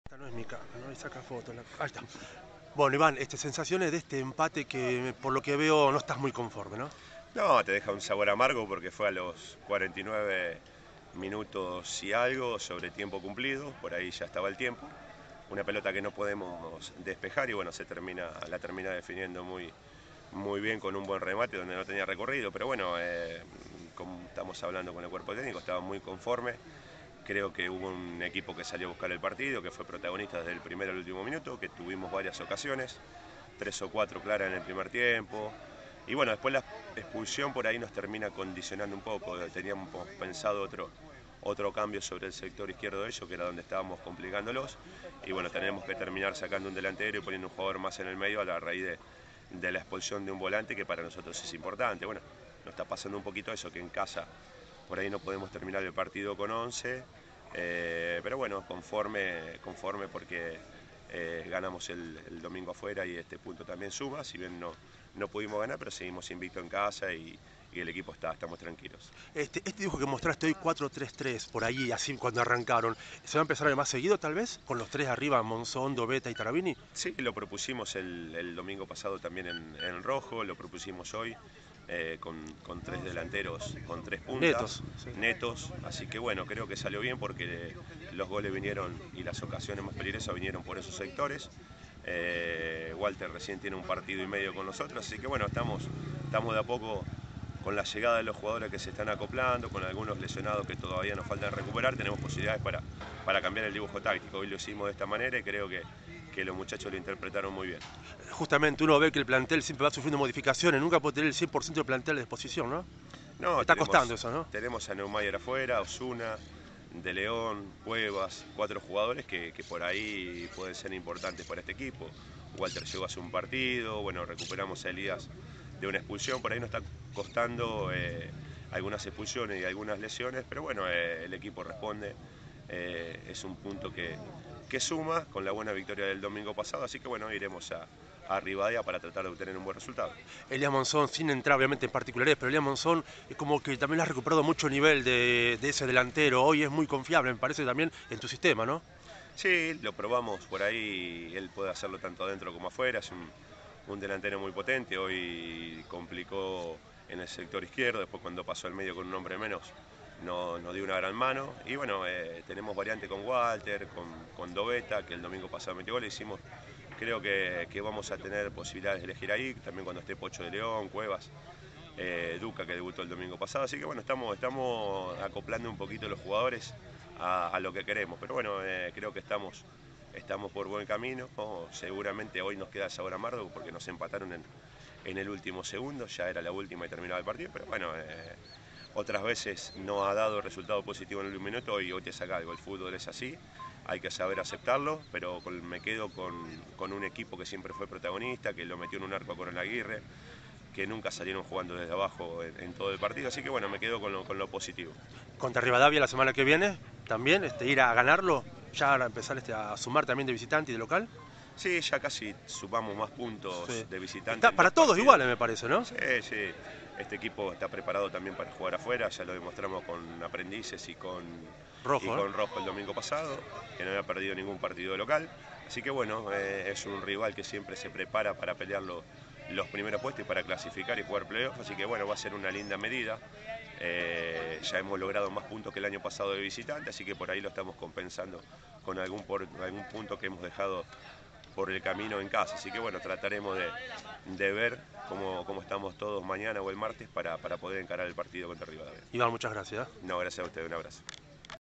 FEDERAL B. IVÁN POTEPÁN DT DE PSM.
La palabra del DT de PSM luego del empate ante Coronel Aguirre.